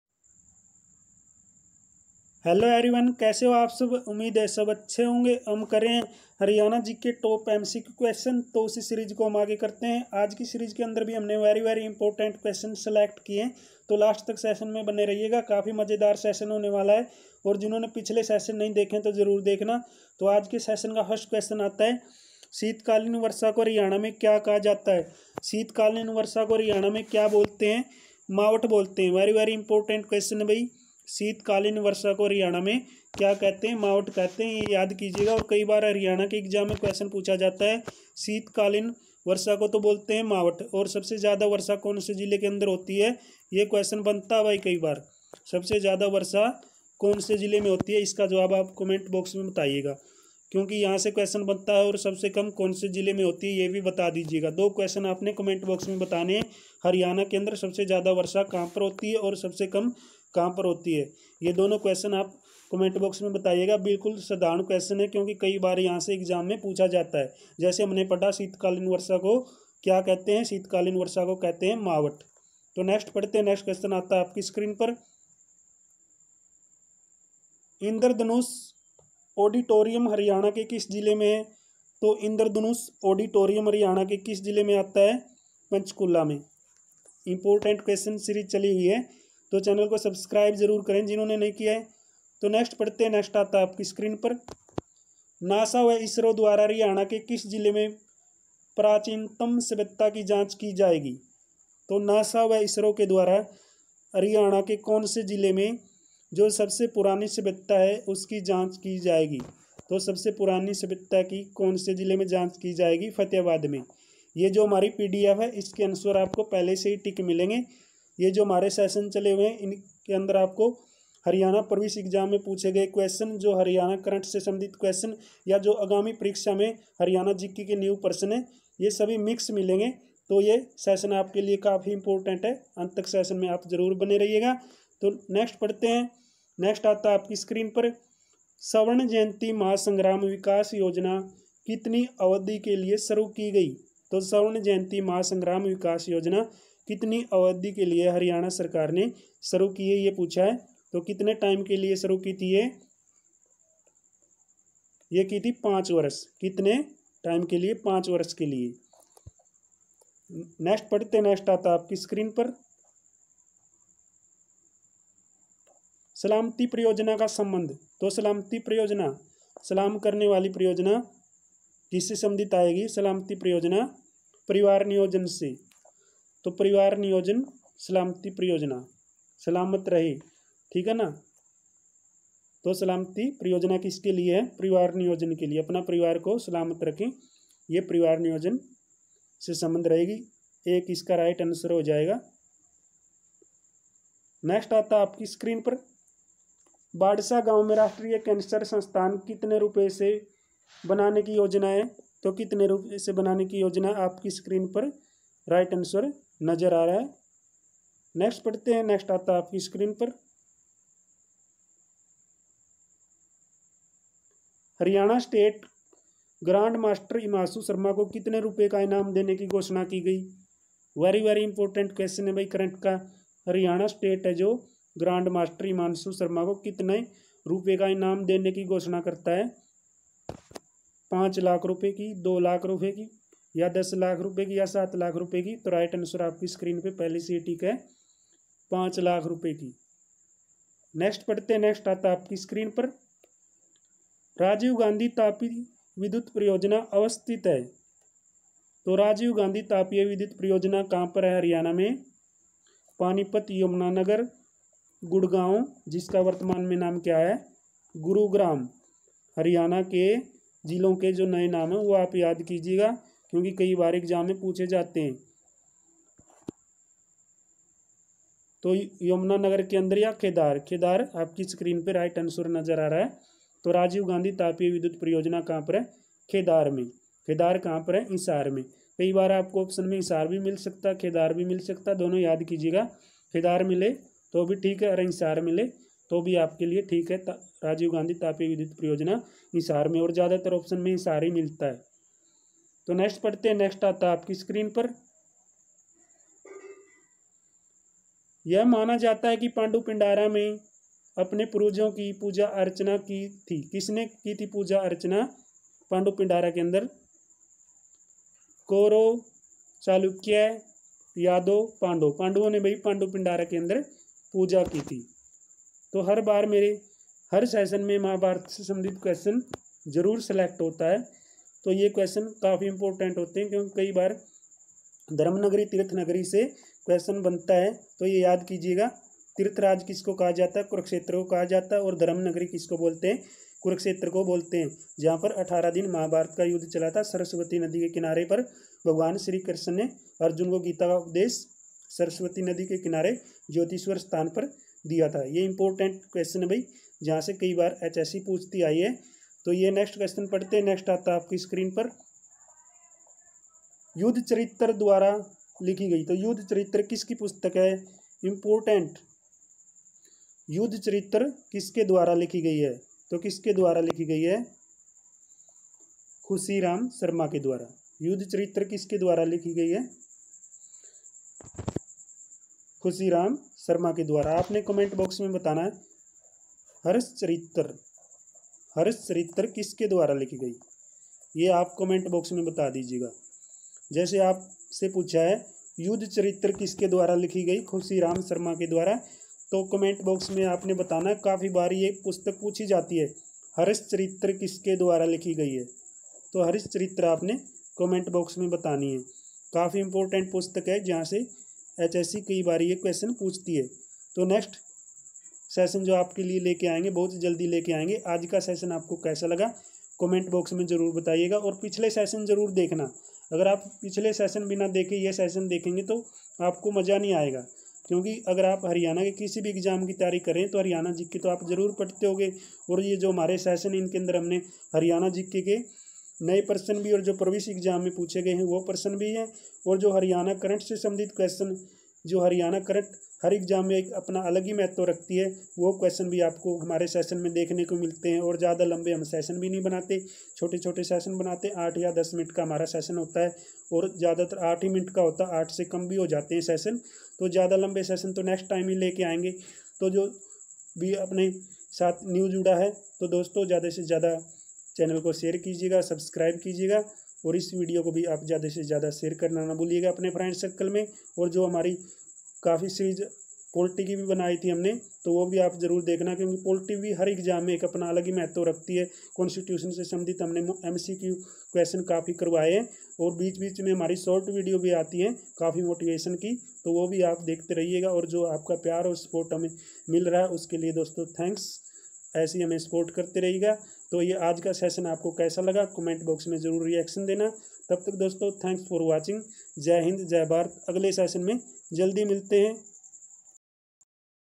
Haryana_Gk | Haryana gk Top Mcq questions Session - 11
आज हम पढेंगे हरियाणा जीके के अति महत्वपूर्ण प्रशन्न ..  Haryana Gk By Anil Sir

विडियो पूरी देखें...